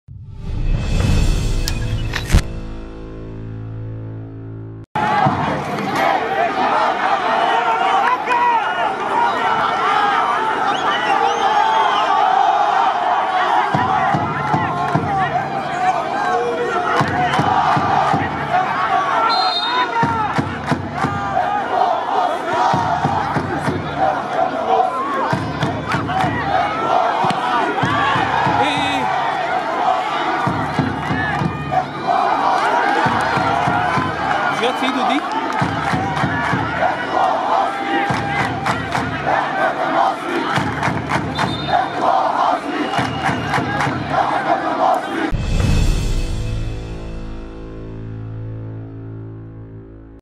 غضب هستيرى على دكة الأهلى فى مباراة البنك... طالبوا الرجوع للفار
غضب هستيرى على دكة الأهلى فى مباراة البك... طالبوا الرجوع للفار